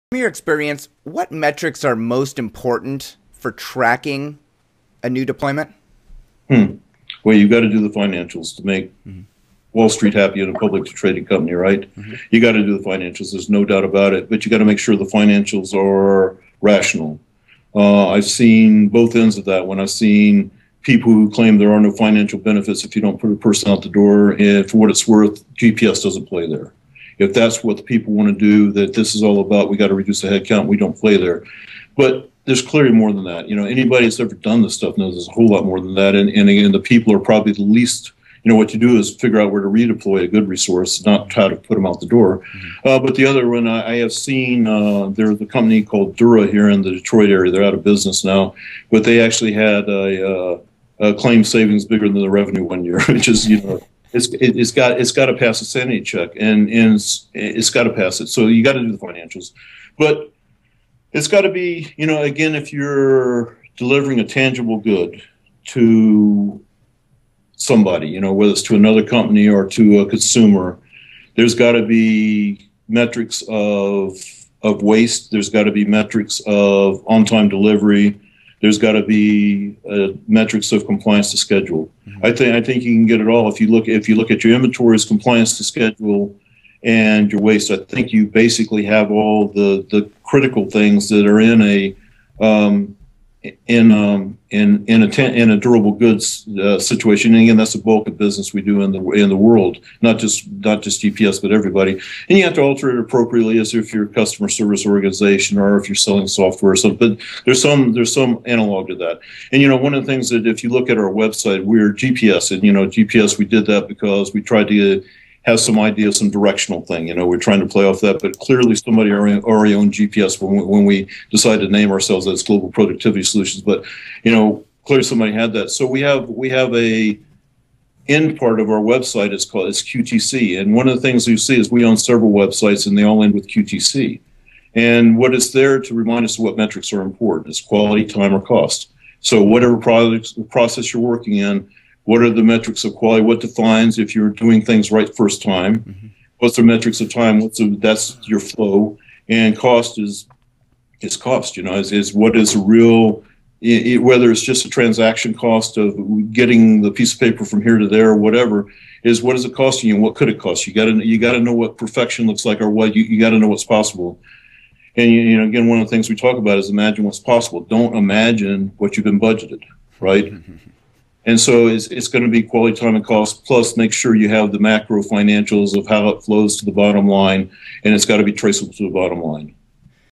What Metrics Are Most Important in a Lean Six Sigma Deployment Plan?
What metrics are most important for tracking a new deployment? Well, you got to do the financials to make Wall Street happy if it's a publicly traded company, right? You've got to do the financials, there's no doubt about it but you got to make sure the financials are rational. I have seen both end of that. When I see people who claim there are no financial benefits if you don't put the person out the door...